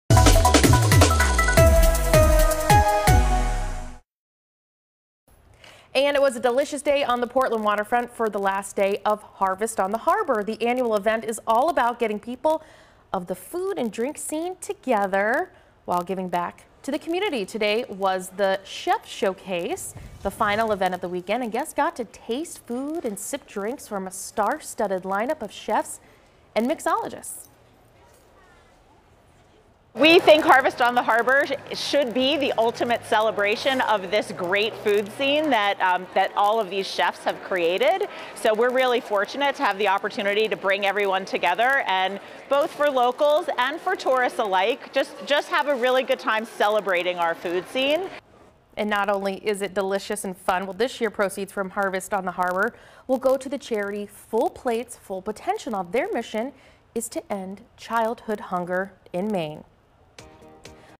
Many participate in Portland's Harvest on the Harbor event
Proceeds from this year's Harvest on the Water event go to the charity "Full Plates Full Potential" whose mission is to end childhood hunger.